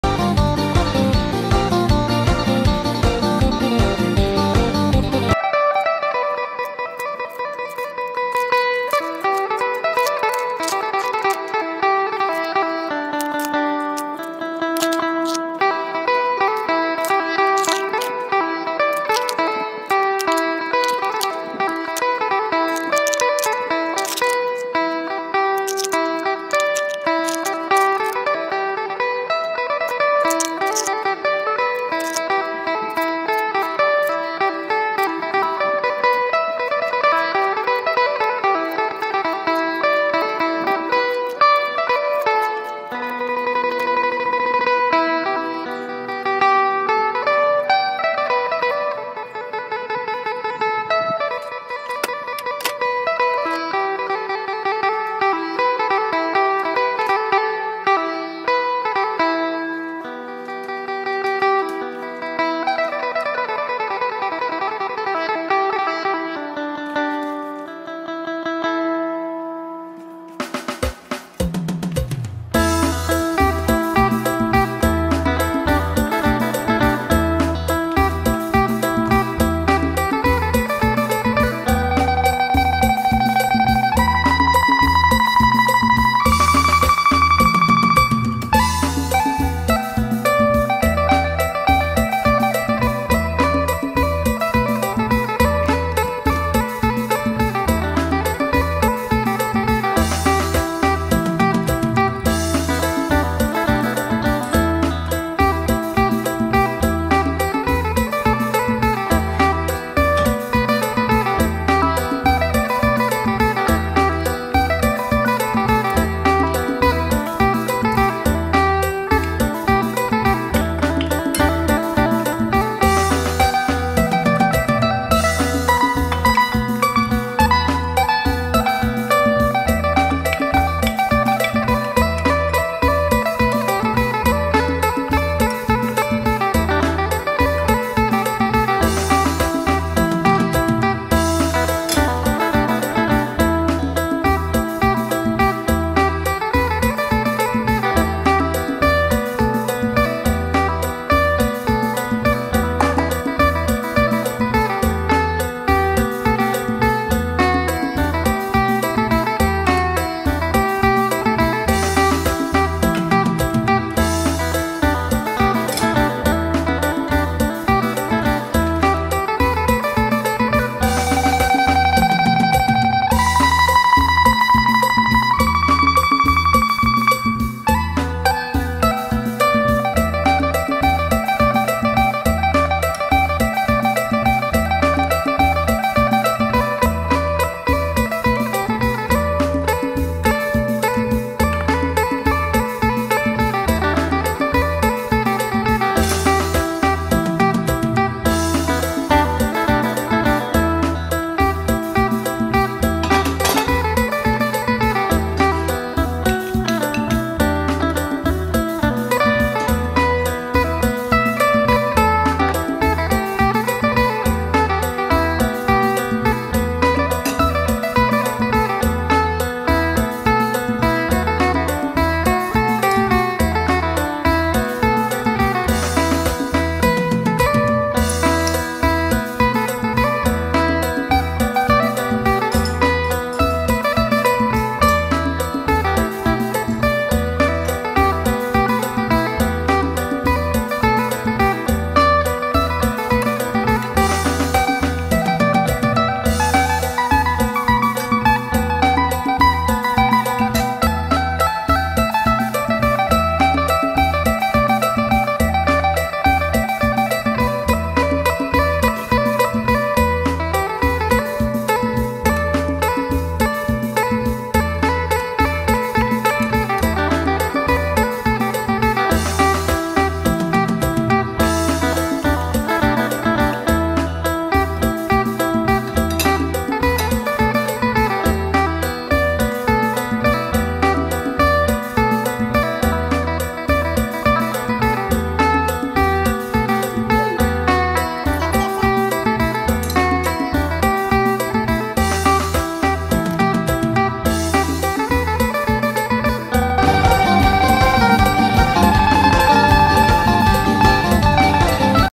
Ep.70 วิธีแกงเห็ดมันปู🍄🍄ใส่หน่อไม้ สไตล์อีสานบ้านๆ /Chanterelle mushrooms TX USA
วิธีแกงเห็ดมันปูใส่หน่อไม้ สไตล์อีสานบ้านๆ@usa5659
เห็ดมันปูสามารถทำอาหารได้หลายเมนูตามต้องการ